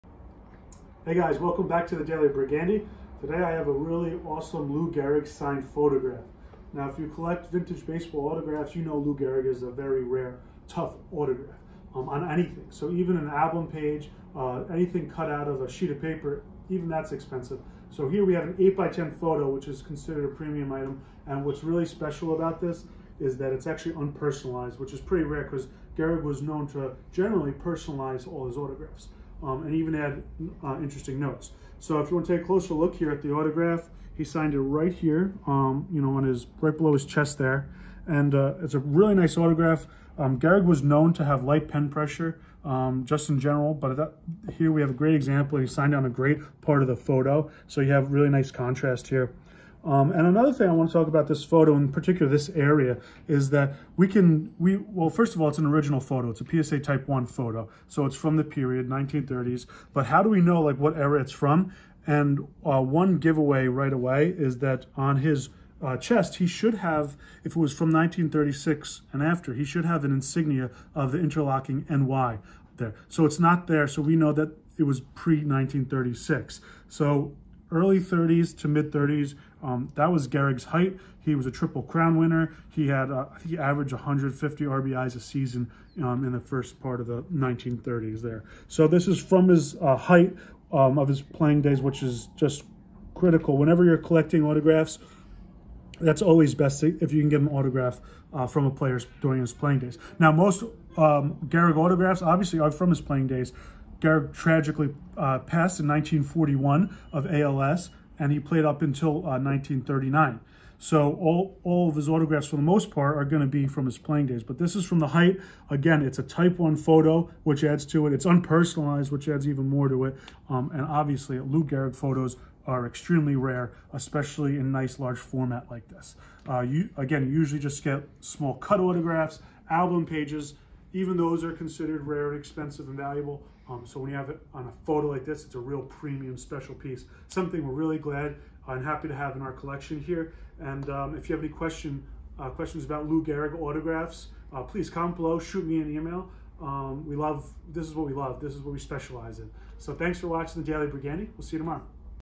1930s Lou Gehrig Signed 8x10 Photo, Type 1. PSA
1930s Lou Gehrig Signed 8x10 Photo, Type 1.
Autographs from the Iron Horse are rare, especially when they’re unpersonalized on a large photograph!!